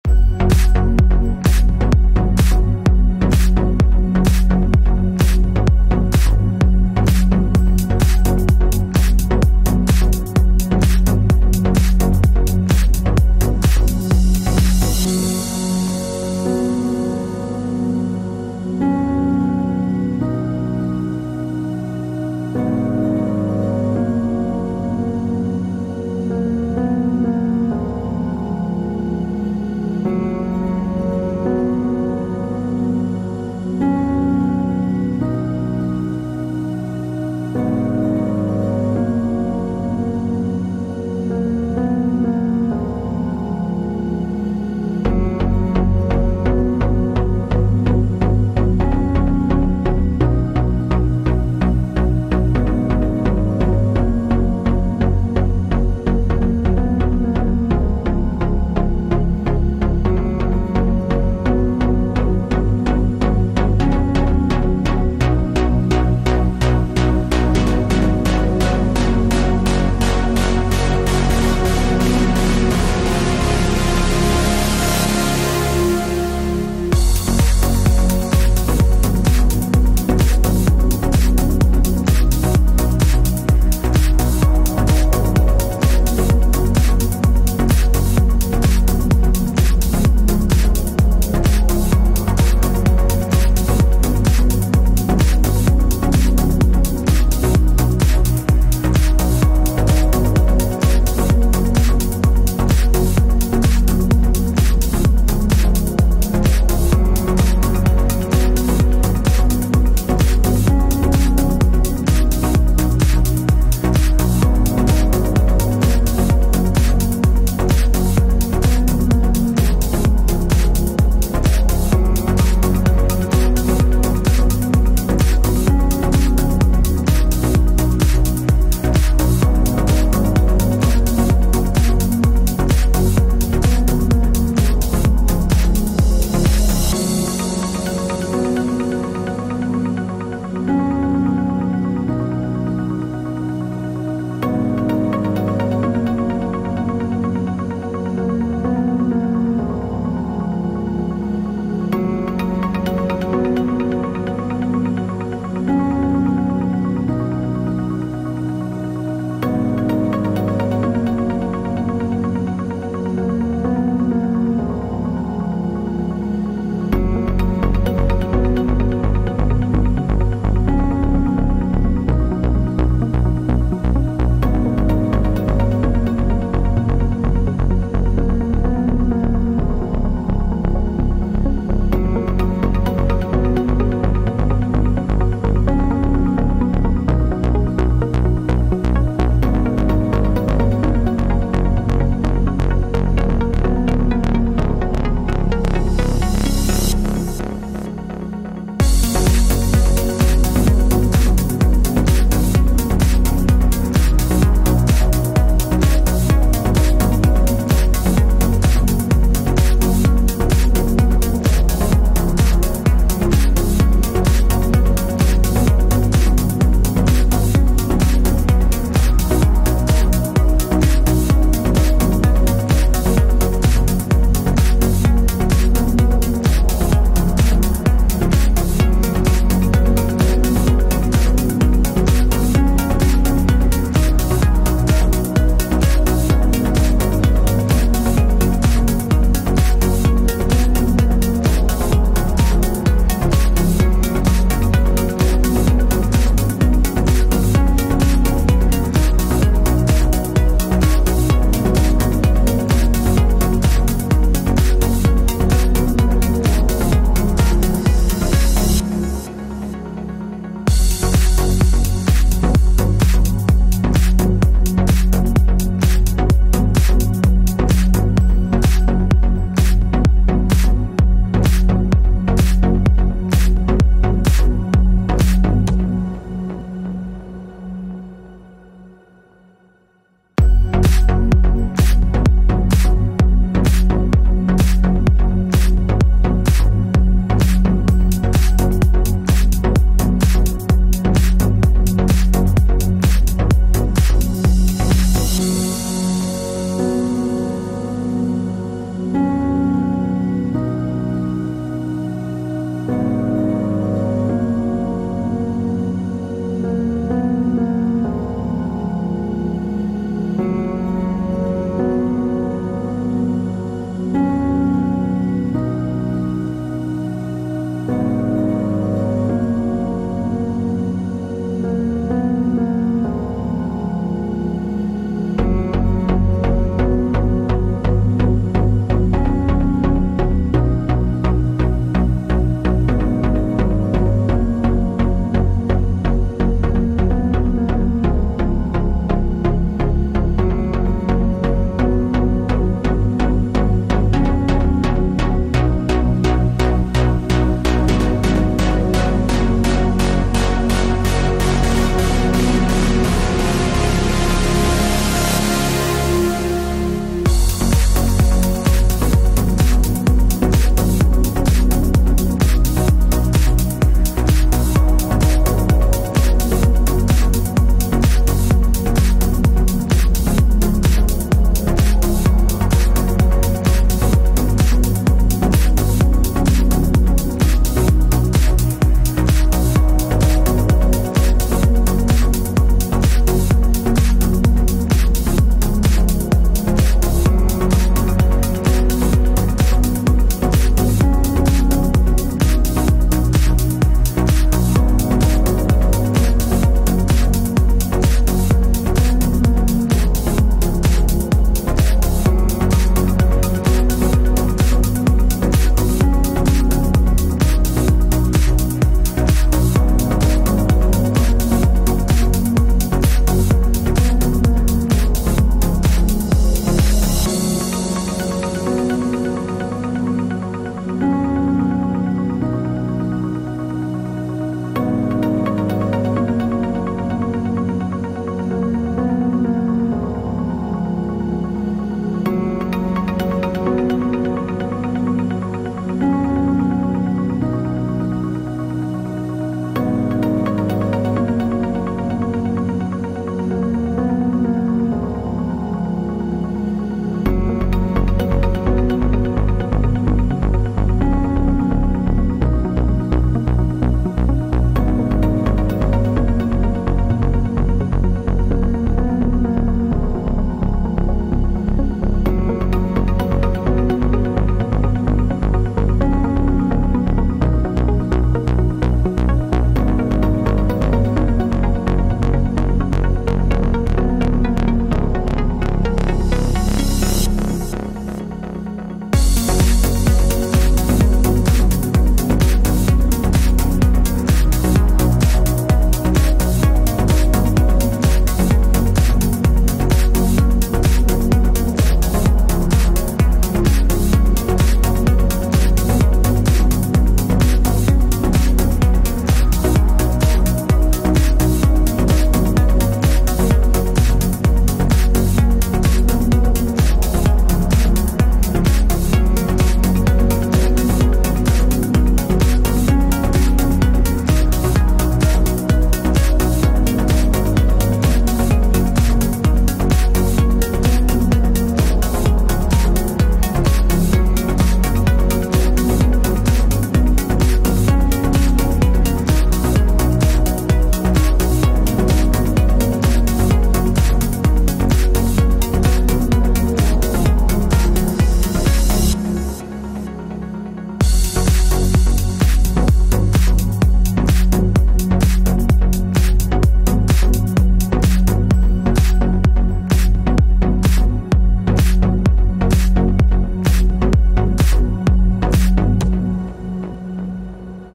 Free Bypass iCloud with Signal Untethered auto block Update OTA - Erase all data & settings
Support:  iPhone 6 to X and iPad Pro, iPad Pro 10.5, iPad mini 3, iPad mini 4, iPad Air 2, iPad Air, iPad 2017, iPad 2018. iOS 12.3 -  to 13.6.1